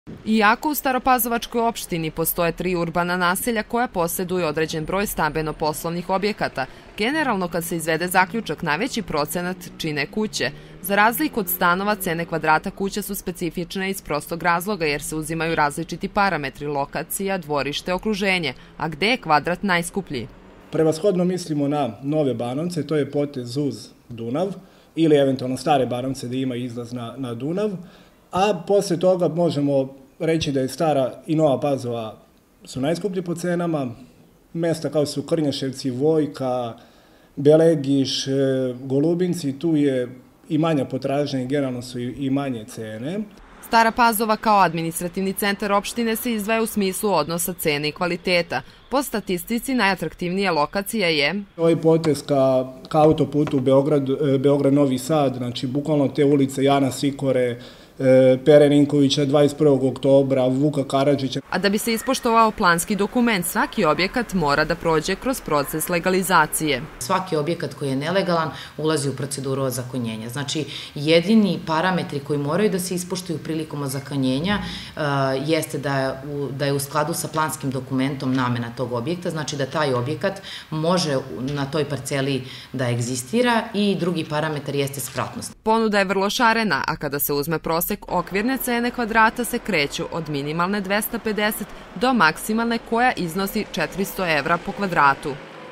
Ponuda i cene kuća
Sa 3 urbana i 7 seoskih naselja od stambenih objekata svakako da prednjače kuće u Opštini Stara Pazova. ponuda je velika, potražnja se razlikuje od mesta do mesta, a cene se kreću od 250 do 400e po kvadratu.